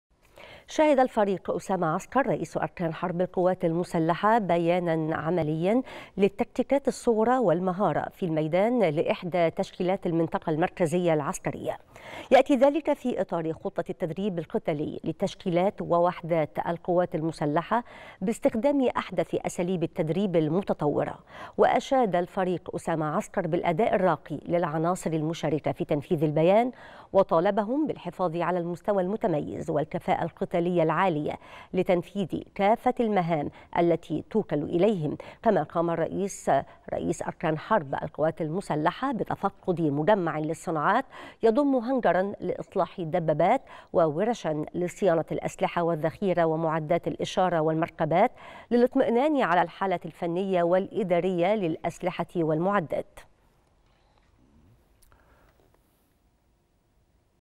رئيس الأركان يشهد بيانا عمليا للتكتيكات الصغرى والمهارة في الميدان لإحدى وحدات المنطقة العسكرية
نشرة الثالثة | رئيس الأركان يشهد بيانا عمليا للتكتيكات الصغرى والمهارة في الميدان لإحدى وحدات المنطقة المركزية العسكرية